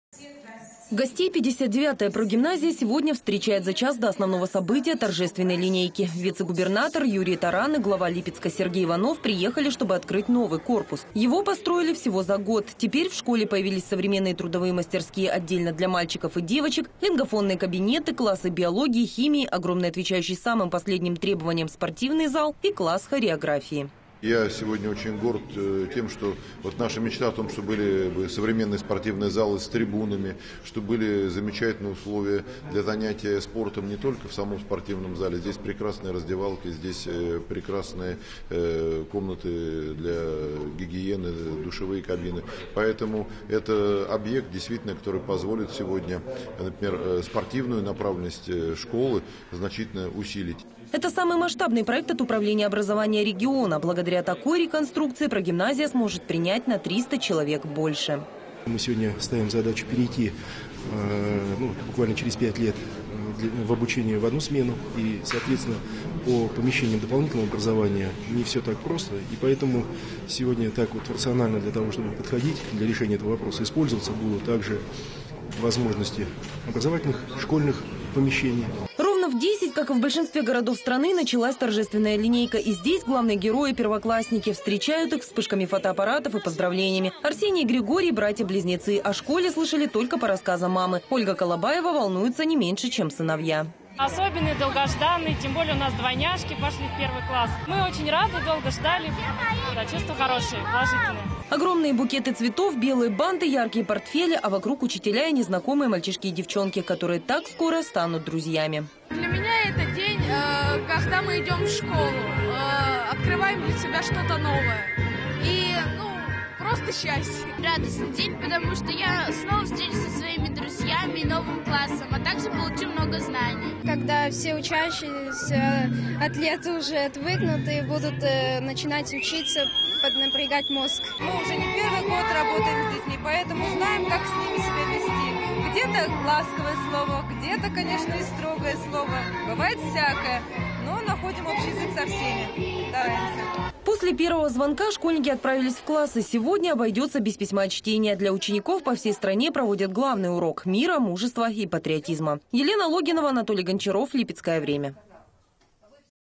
Новый корпус открыли в прогимназии №59 "Персп...
Новый корпус открыли в прогимназии №59 "Перспектива"
 Новый корпус открыли в прогимназии №59 &quot;Перспектива&quot; _